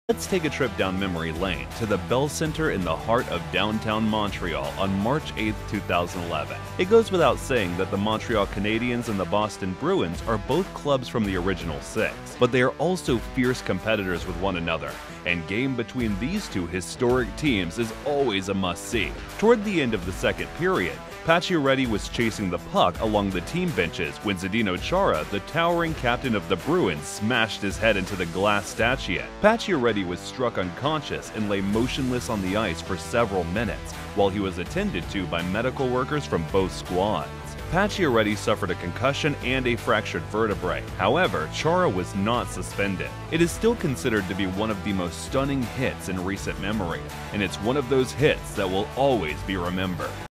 The Bone-Crushing Hit That Shook the Ice - Chara vs Pacioretty
In this video, we take a look at one of the most bone-crushing hits in NHL history, when Zdeno Chara came down hard on Max Pacioretty.